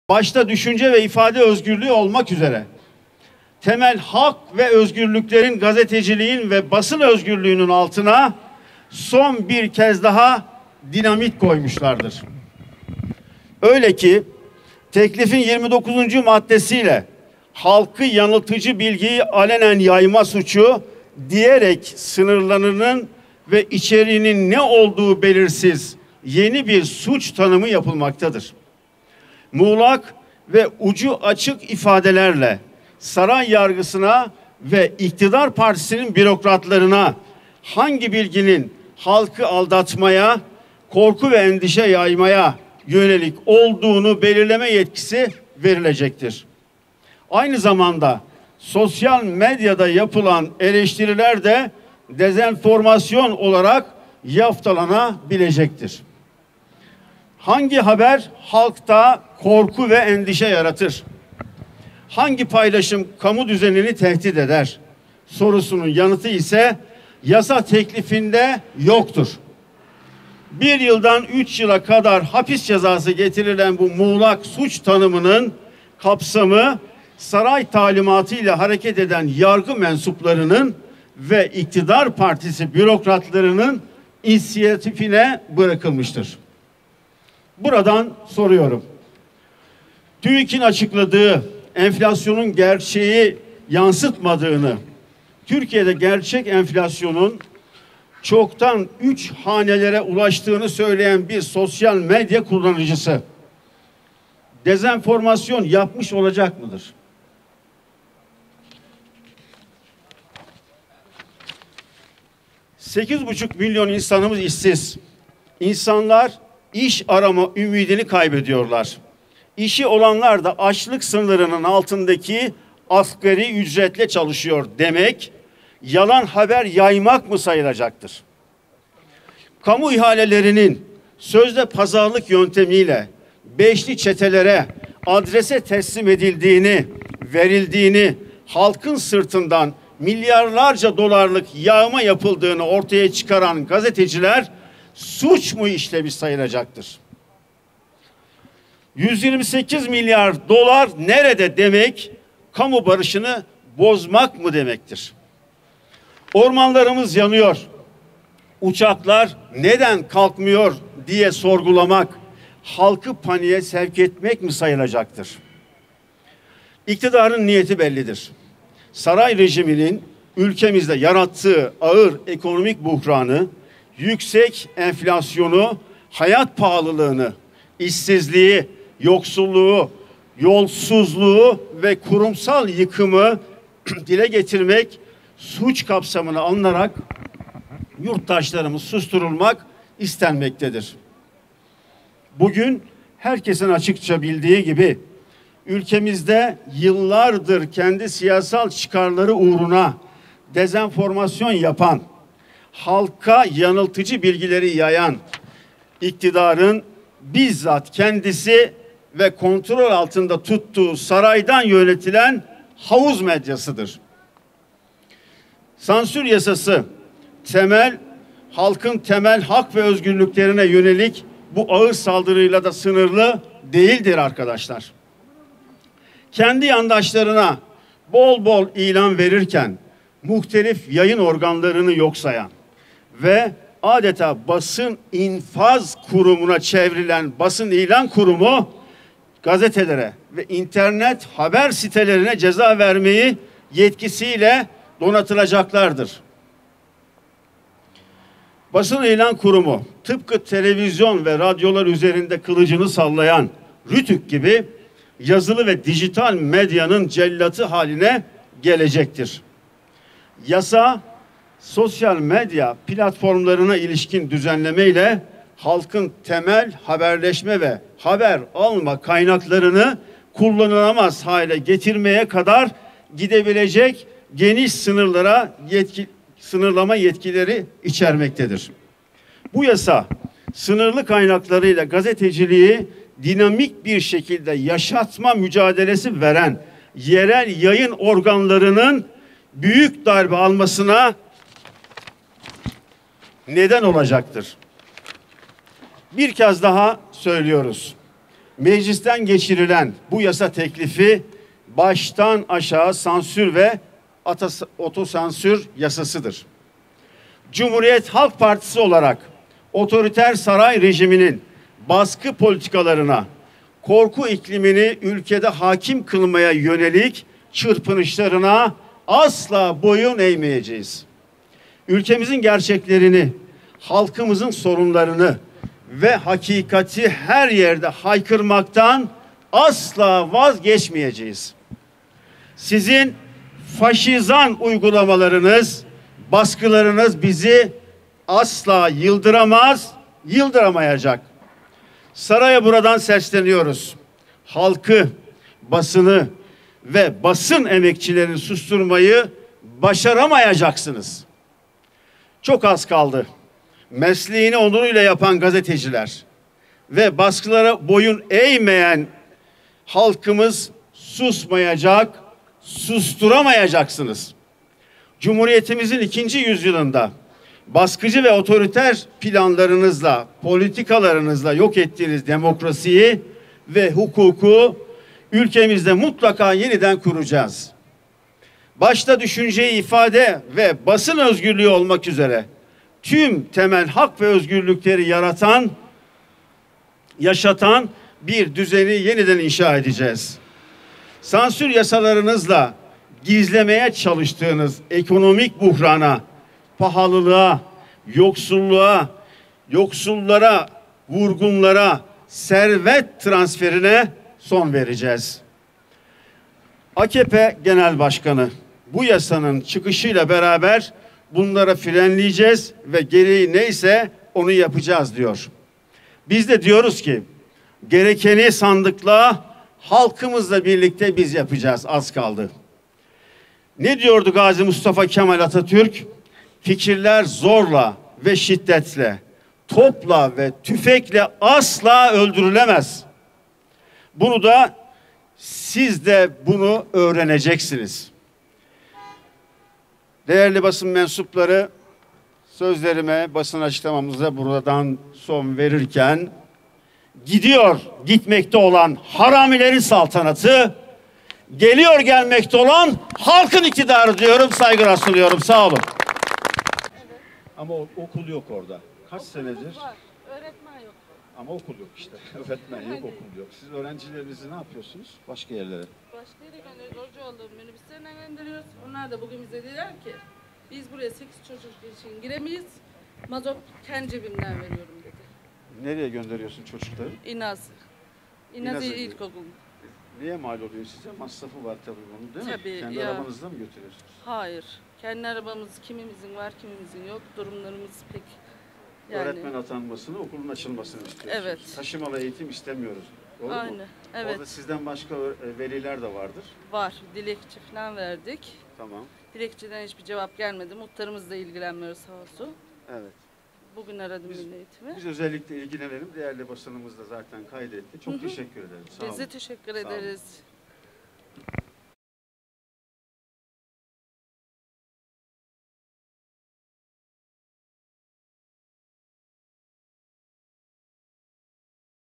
CHP’li Görgöz: Bu yasa baştan aşağı sansürdür!
CHP İl Başkanı Yalçın Görgöz, Zafer Meydanı’nda yaptığı basın açıklamasında TBMM’de yasalaşan Dezenformasyon yasası hakkında konuşarak, “Bu yasa teklifi, baştan aşağı sansür ve otosansür teklifidir. Mesleğini onuruyla yapan gazeteciler ve baskılara boyun eğmeyen halkımız susmayacak, susturamayacaksınız” dedi